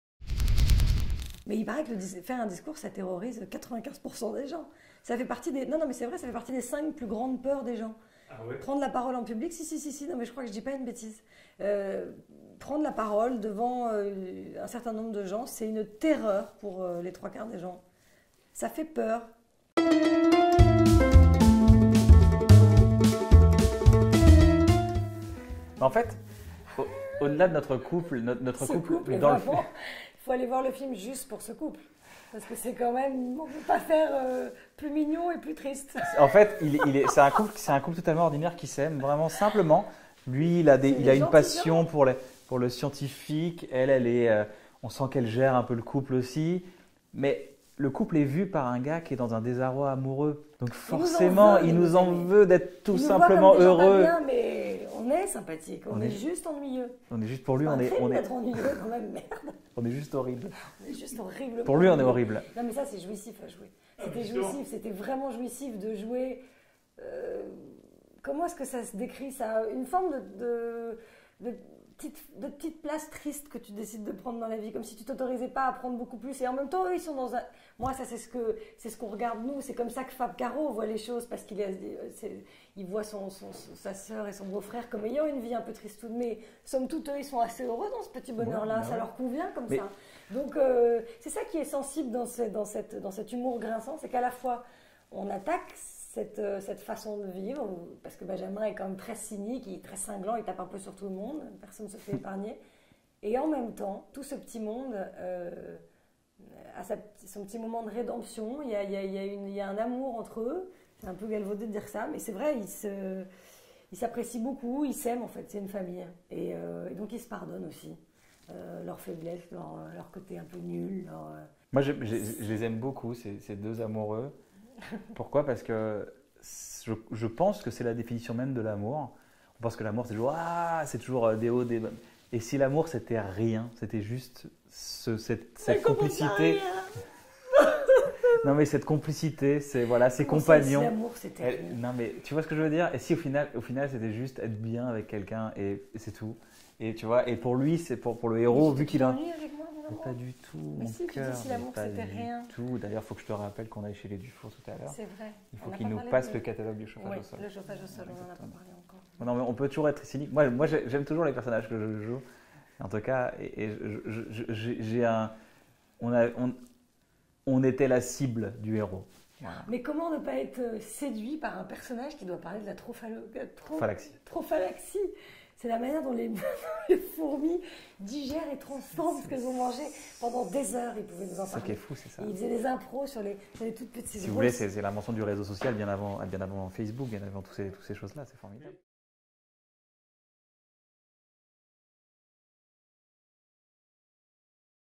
Kyan Khojandi et Julia Piaton : leur couple dans « Le Discours » // Extrait archives M6 Video Bank
« Et si l’amour, c’était rien ? »

Pour la sortie du film « Le Discours », Julia Piaton et Kyan Khojandi, en couple dans le film, nous donnent leur vision de leur amour à l’écran, et il n'ont pas l’air toujours d’accord ! 😂